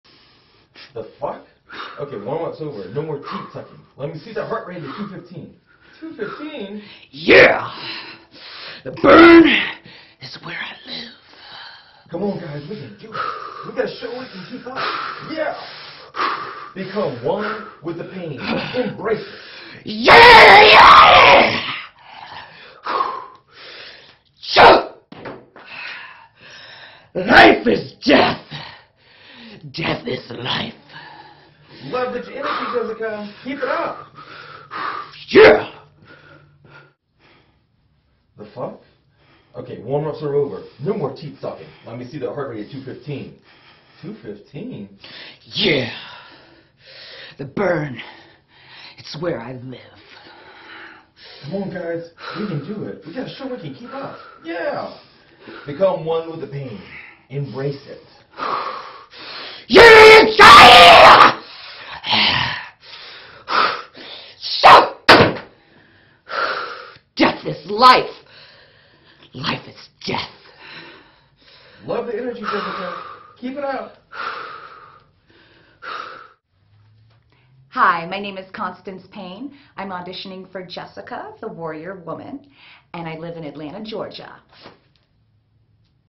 Warrior Woman Audition
This is an audition I did for Respectful Productions last year. They were looking for 3 different types of wild characters for their comedy bit, so I read all the lines for each character and gave them 2 very different takes.

I got the part. They even put me in another scene for that bit and I improved a line that they thought was AWESOME, so it made the final cut.

This small role turned into a big opportunity for me, and their company has had me back several times since.

Check out Respectful Productions on YouTube & Facebook.